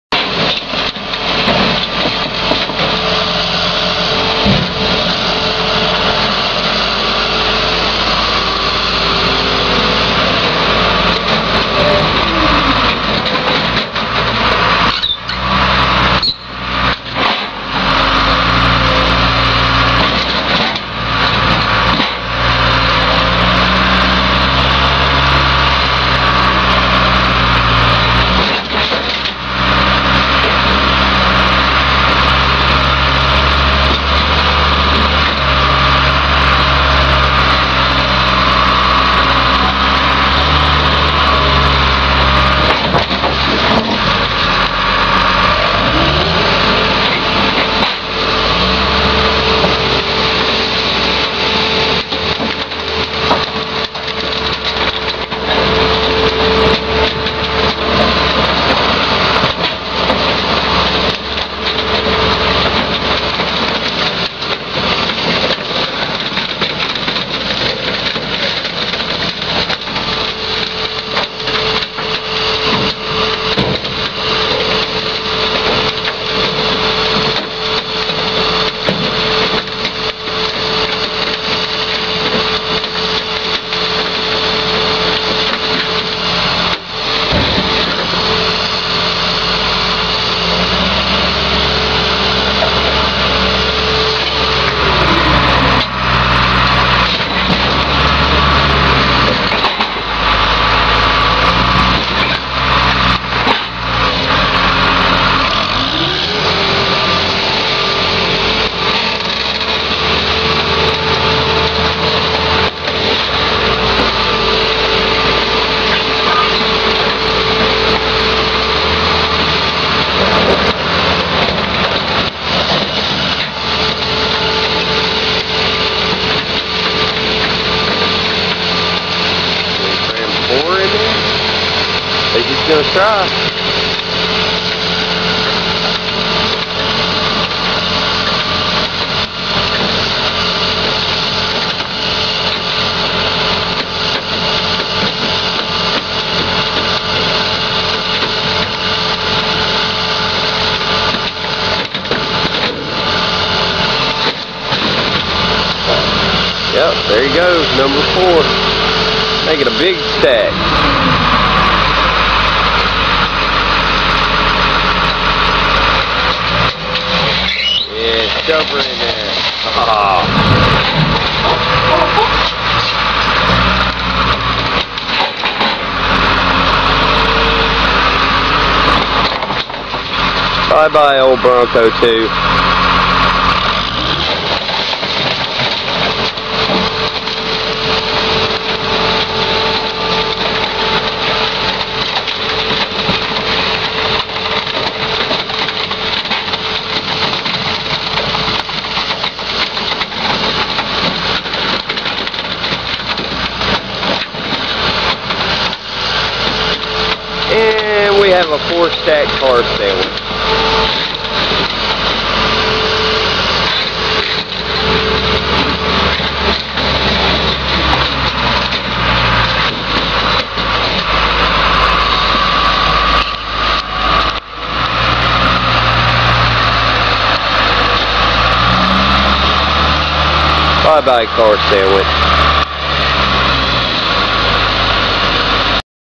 HarrisAuto Salvage 4 car stack in crusher
heres a car crusher crushing car i did not make this video